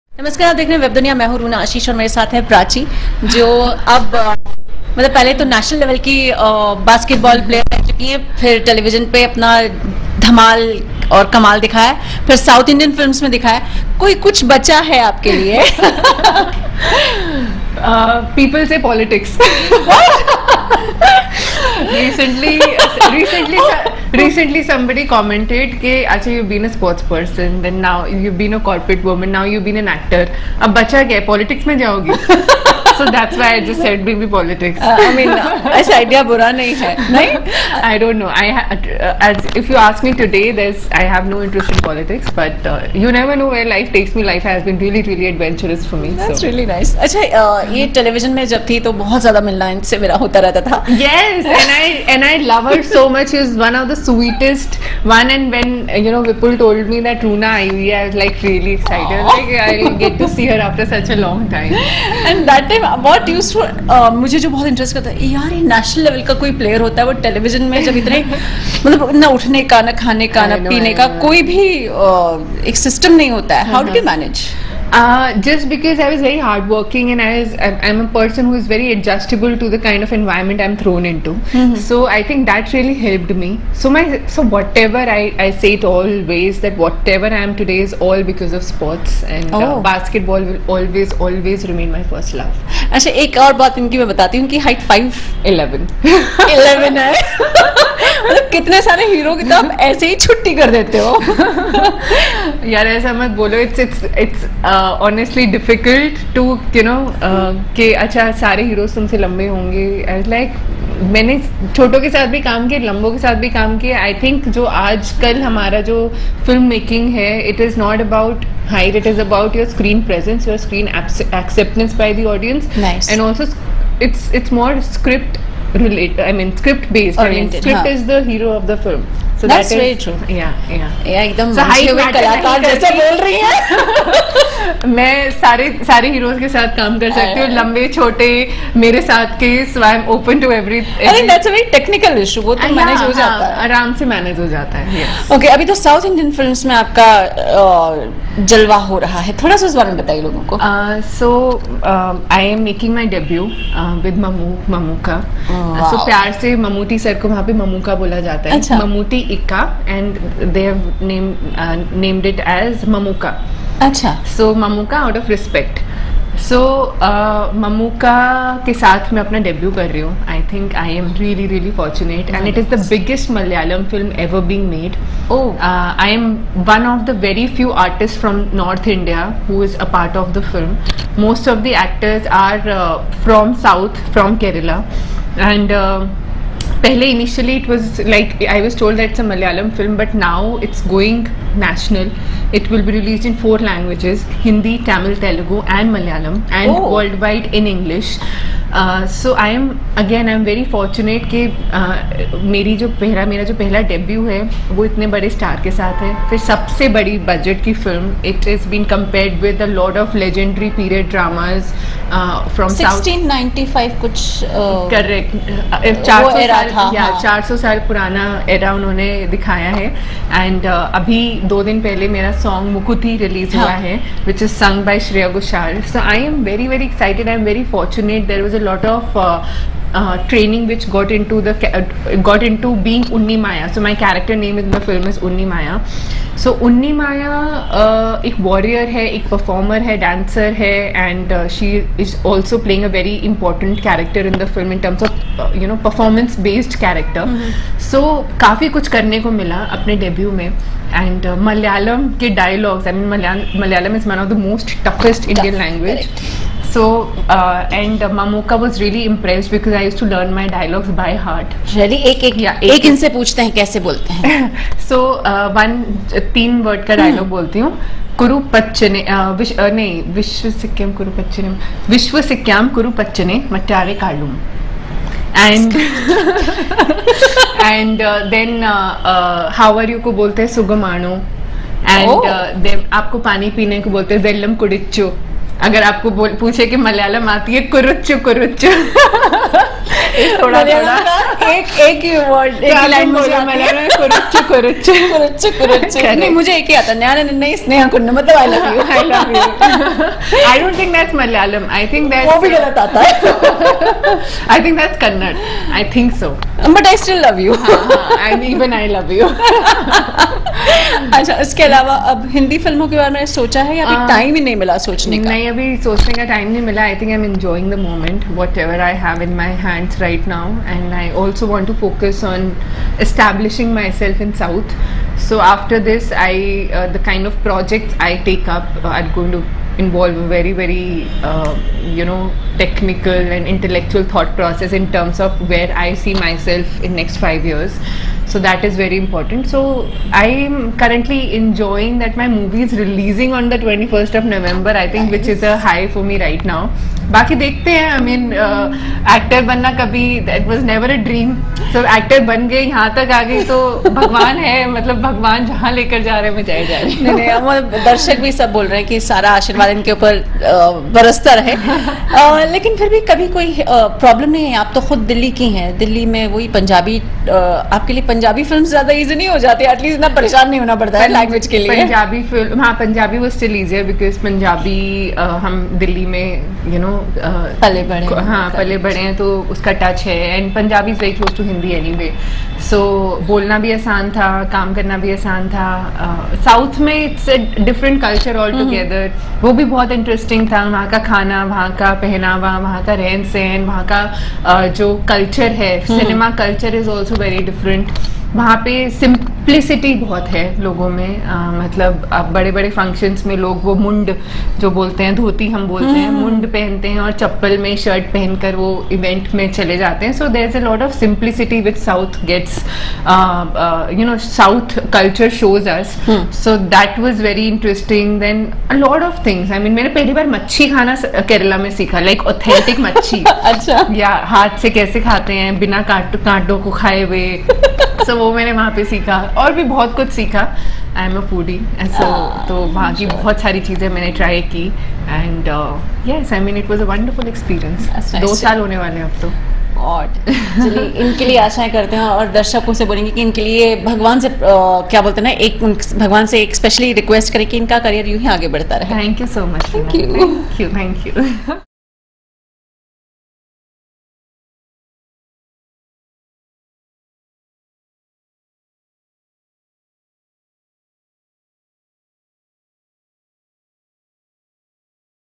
ममूटी के साथ फिल्म कर रही हैं प्राची तेहलान | Mammootty | Mamangam | Prachi Tehlan
बास्केटलबॉल की नेशनल प्लेयर की प्राची तेहलान टीवी में काम कर चुकी हैं। पंजाबी फिल्म कर चुकी हैं। अब वे दक्षिण भारतीय फिल्म कर रही हैं।

वे कहती हैं- ममूटी सर के साथ 'मामंगम' में डेब्यू कर रही हूं। मैं भाग्यशाली हूं जो इस फिल्म का हिस्सा हूं। यह फिल्म 4 भाषाओं में रिलीज होगी। फिल्म में 1695 का समय दिखाया गया है।

फिल्म में मेरा रोल योद्धा और डांसर का है। इसमें मुझे बहुत कुछ करने को मिला है। मलयालम कठिन भाषा है, लेकिन मैंने याद किए। अब मैं कुछ वाक्य सीख गई हूं।